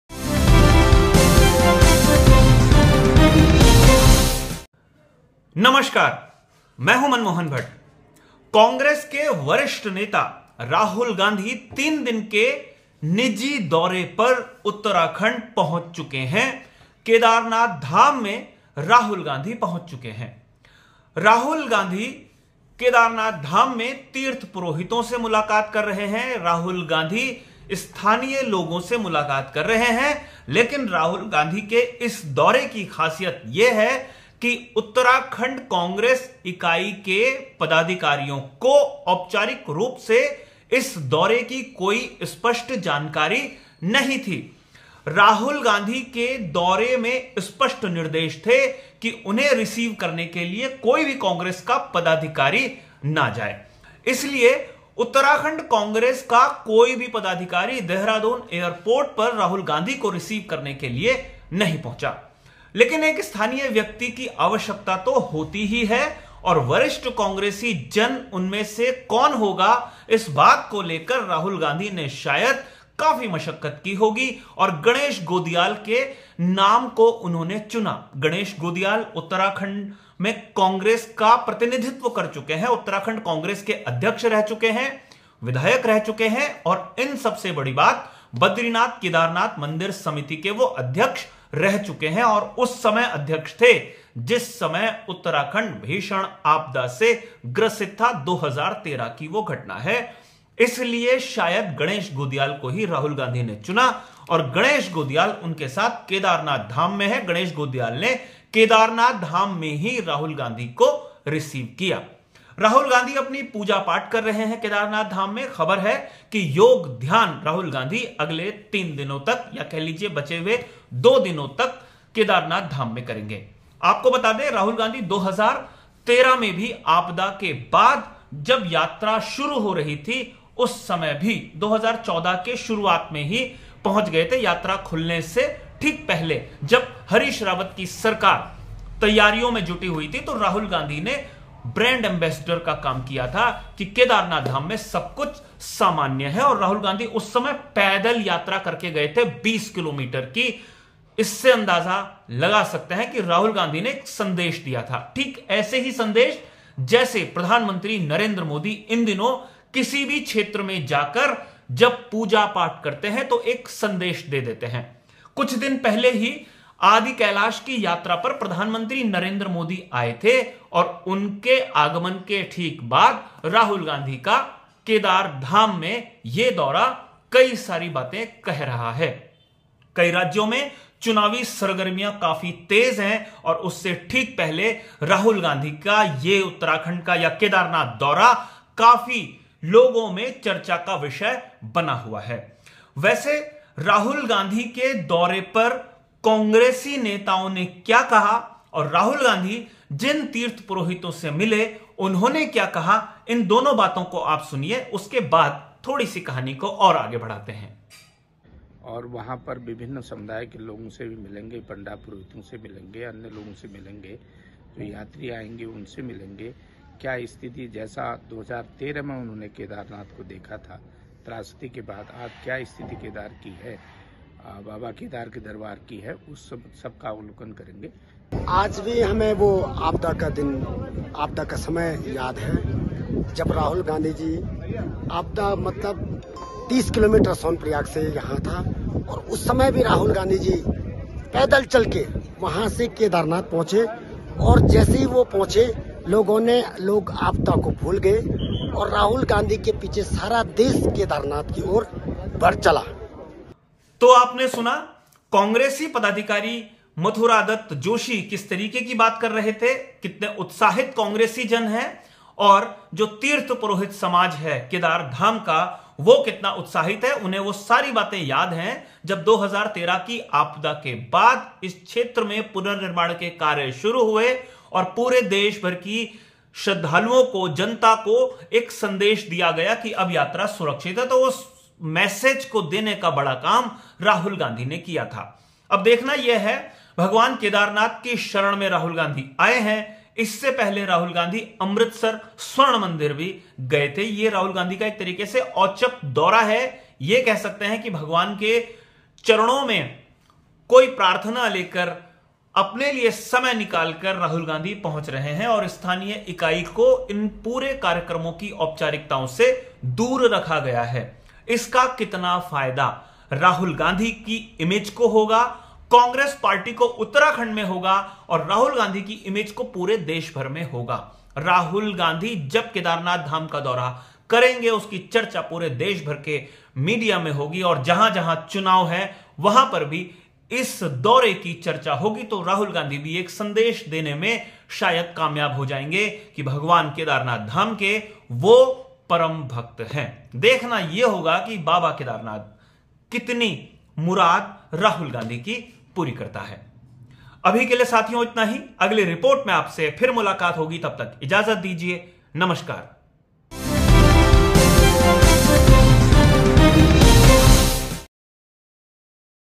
Uttarakhand.Kedarnath धाम से क्या आशीर्वाद मांगा Congress नेता राहुल गांधी ने?Manmohan_Bhatt
कांग्रेस(Congress )की नेता राहुल गांधी (Rahul Gandhi) अचानक उत्तराखंड (Uttarakhand ) दौरे पर पहुंच गए.

 उत्तराखंड कांग्रेस (Uttarakhand Congress) इकाई को इस दौर की भनक भी नहीं लगी.

 राहुल गांधी तीन दिन तक केदारनाथ (Kedarnath ) धाम में योग (yoga)  ध्यान करेंगे .बाबा केदारनाथ धाम की पूजा अर्चना करेंगे.

 राहुल गांधी के साथ कांग्रेस का प्रदेश संगठन का कोई भी पदाधिकारी साथ नहीं है.

 लेकिन राहुल गांधी के साथ बद्रीनाथ केदारनाथ मंदिर समिति (Badrinath kedarnath mandir samiti) के पूर्व अध्यक्ष गणेश गोदियाल (Ganeeh Godiyal) साथ रहेंगे.

 गणेश गोदियाल कांग्रेस के प्रदेश अध्यक्ष भी रह चुके हैं।

 राहुल गांधी ने बाबा केदारनाथ धाम से क्या मांगा?
 राहुल गांधी के इस दौरे का क्या राजनीतिक संदेश है?

 देखिए इस रिपोर्ट में.